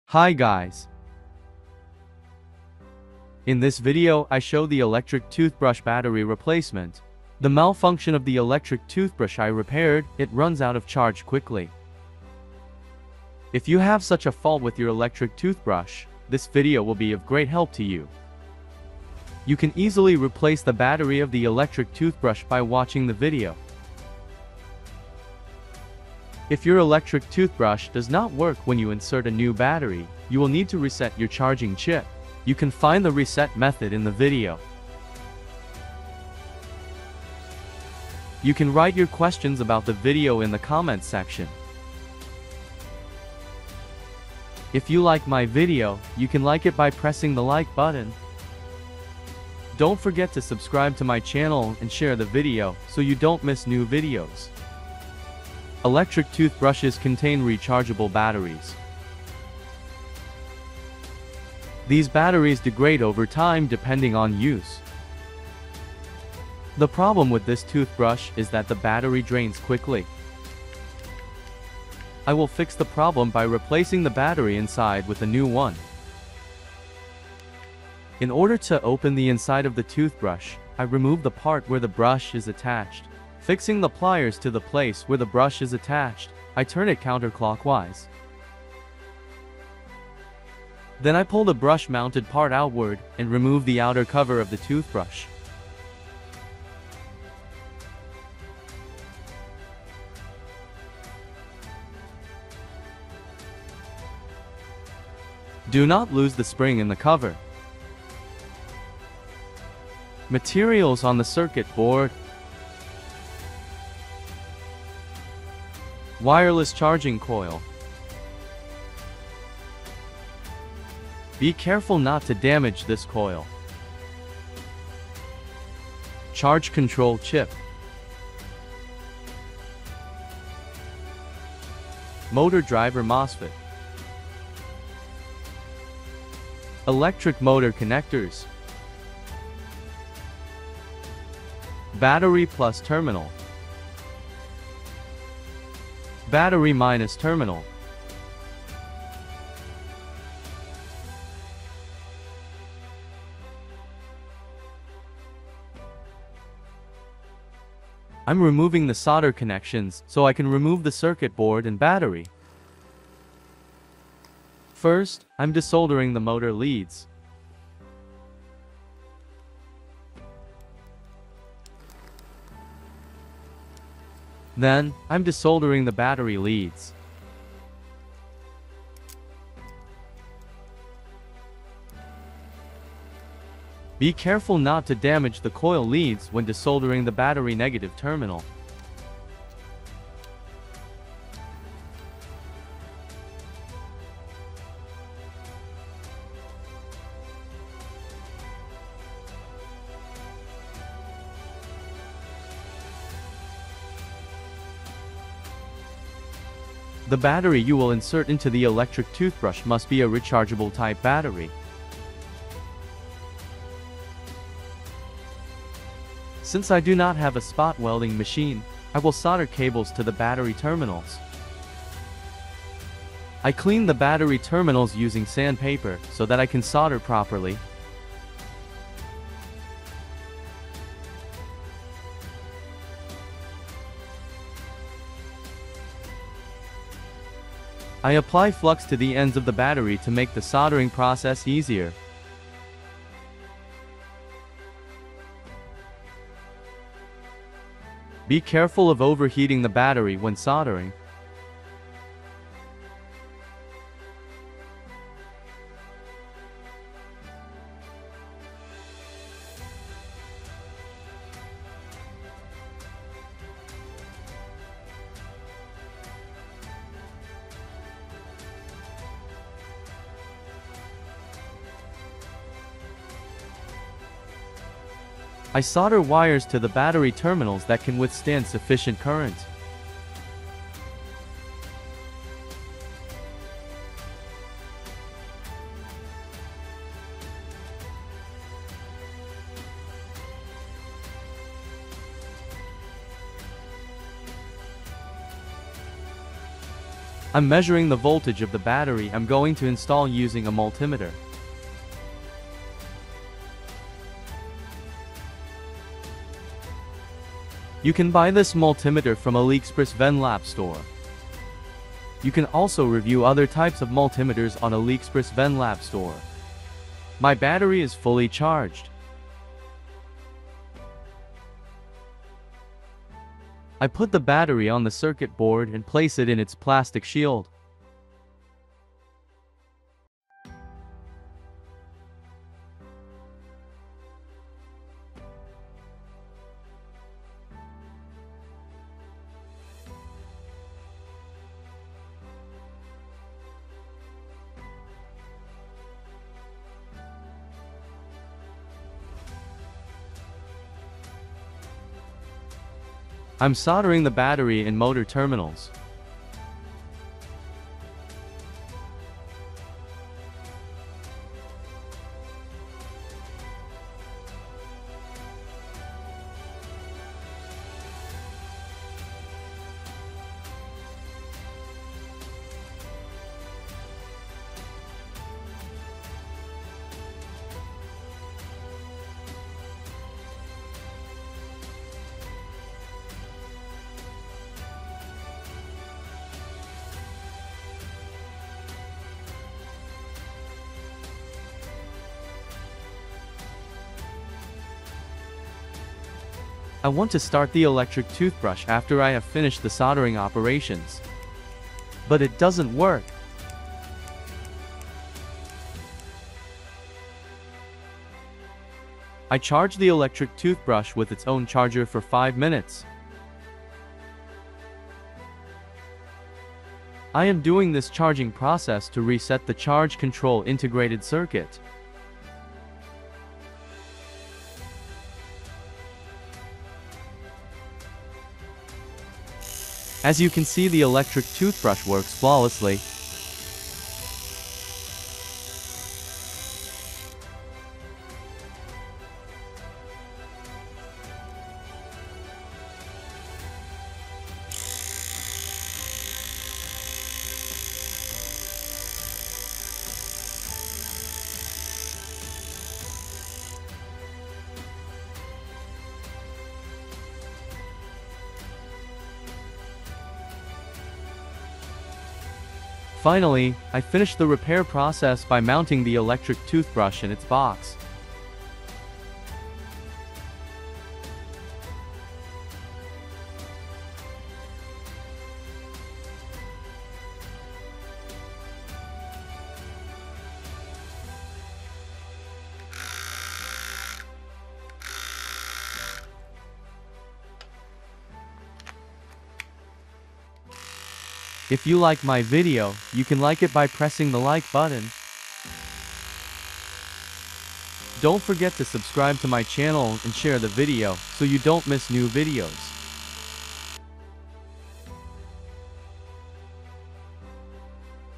Don't Throw Away Your Faulty Electric Toothbrush Without Watching This Video.
Electric toothbrush repair.Electric toothbrush battery replacement.Hello friends.  In this video, I am replacing the battery of an electric toothbrush that runs out of charge quickly.  Electric toothbrushes contain rechargeable batteries.  This battery degrades over time depending on usage.  The most important point to consider when replacing the electric toothbrush battery is that the battery we will install is also a rechargeable battery.  Since I don't have a spot welder, I soldered a cable to the pole of the battery.  When soldering the batteries, we must be careful that the battery does not overheat.  I recommend watching this video before throwing away your electric toothbrush.

3TB5OID0CWKM

My other repair videos.

Do not forget to "like" the video if you like it.

00:00 Video summary.
 00:55 Malfunction of electric toothbrush.
 01:18 How to disassemble an electric toothbrush.
 01:51 Materials on the electronic circuit board.
 02:30 Electronic circuit board and battery disassembly.
 03:27 New battery pack.
 07:09 Electric toothbrush test.